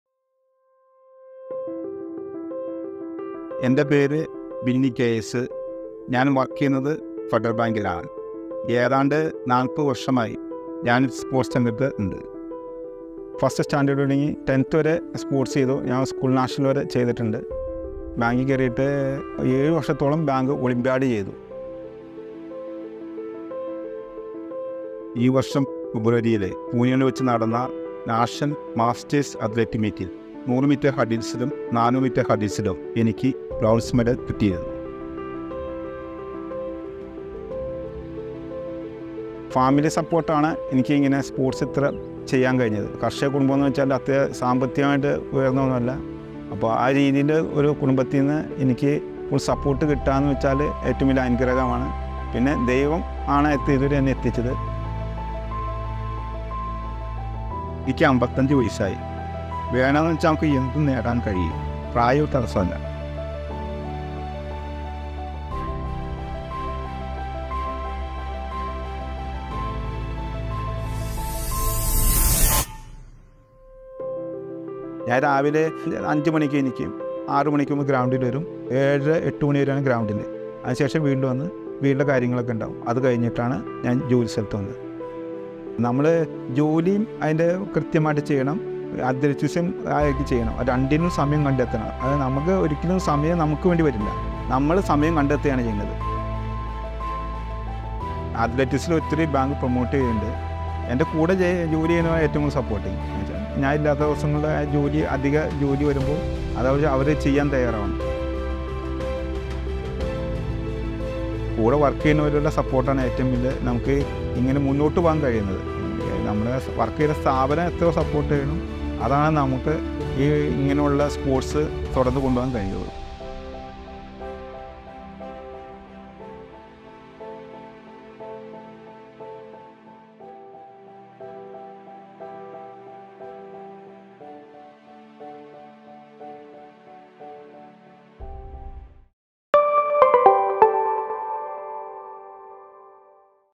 Who said life is full of hurdles?
Life could be full of hurdles. But with a little resilience and a lot of hard work, Binny K S, a fifty-five-year-old employee of Federal Bank, leaped over them and won bronze for 100 & 400 metre hurdles at the National Masters Athletic Meet held in Pune earlier this year. We are super proud of Binny and continue to be inspired by his journey.